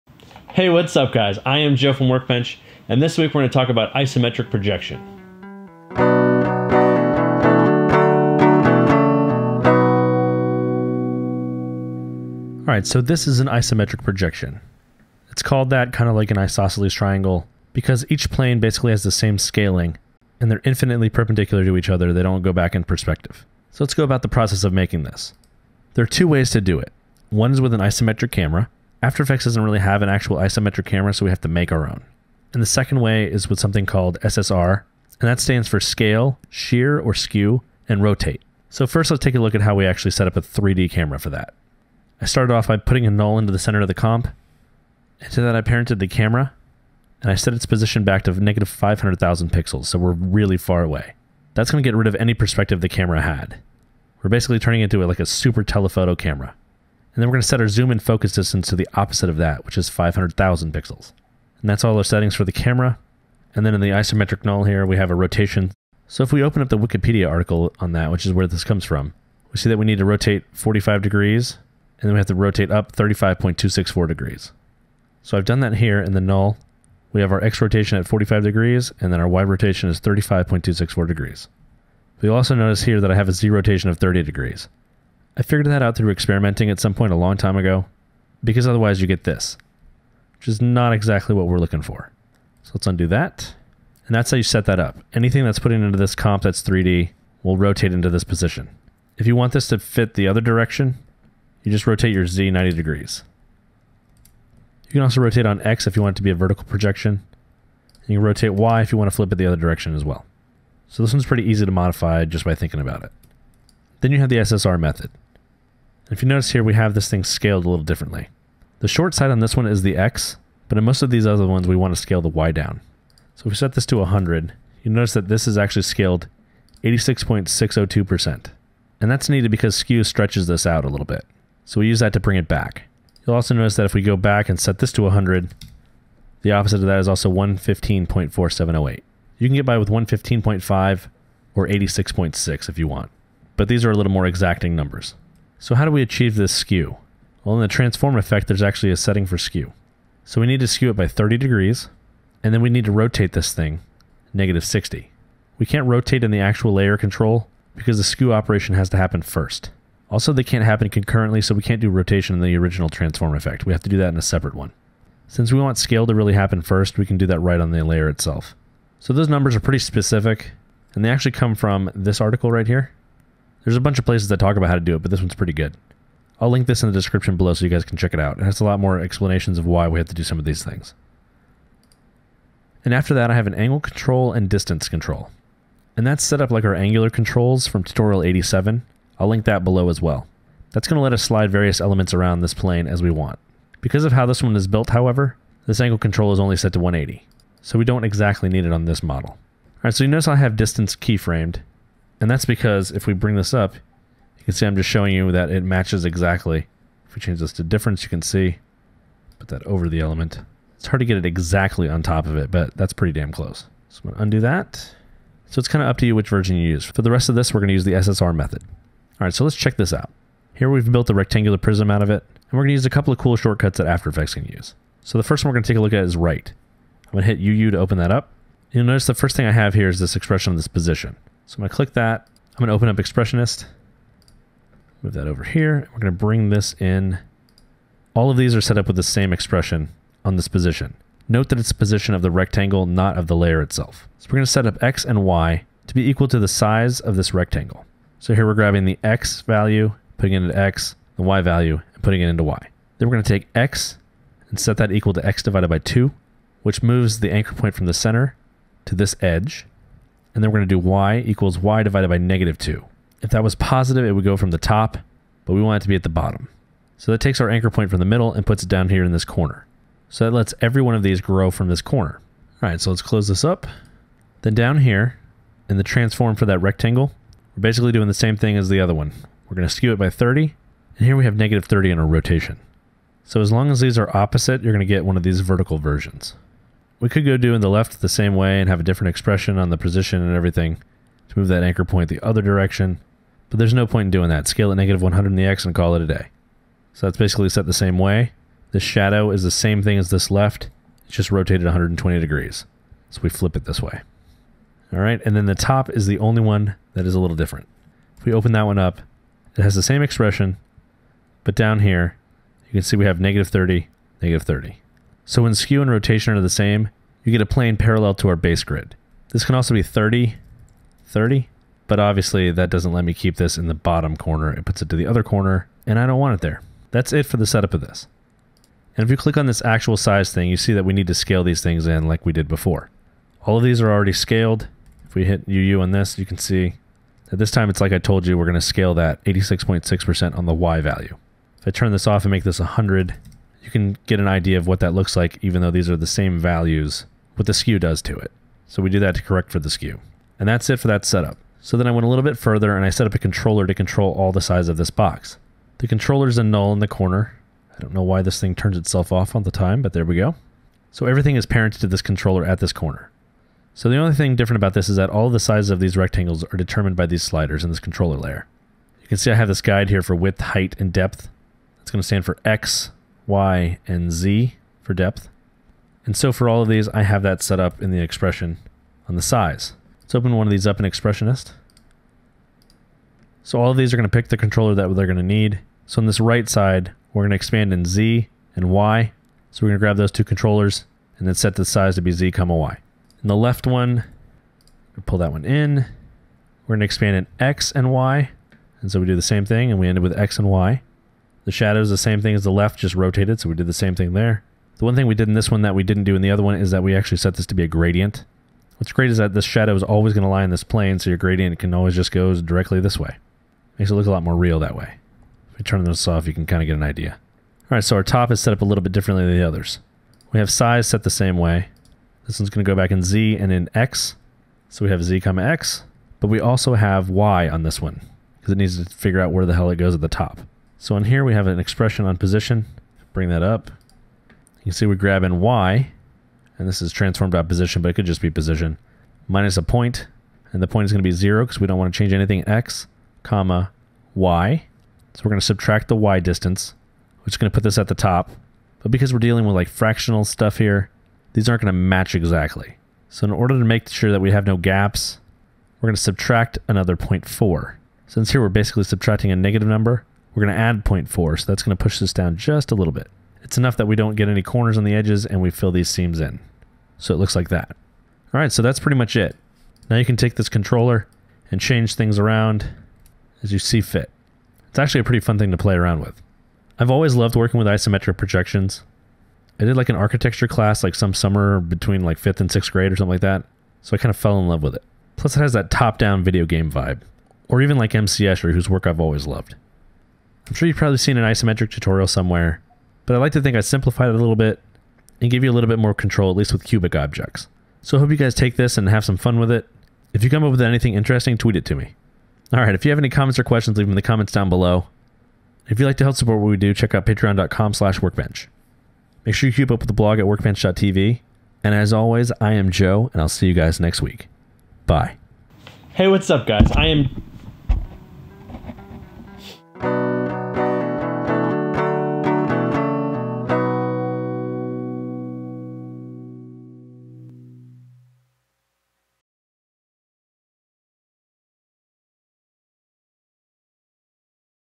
Tutorial 103: Isometric Projection
How to achieve isometric projection inside of After Effects

This week we take a look at how to make an isometric projection by either faking an isometric camera in After Effects, or using the SSR—Scale, Shear or Skew, and Rotate—method.

We also rig up a cubic object so that we can control it with sliders. You can use this to build anything cubic, like buildings. And I forgot to mention it during the tutorial, but if you duplicate the sides and make them smaller, you can combine them with a repeater to get windows!

For more information about the math involved, check out this Tuts+ Illustrator tutorial and be sure to read the comments as well. And you might also find our Tutorial 87: Angular Controls to be helpful when moving things along the isometric grid.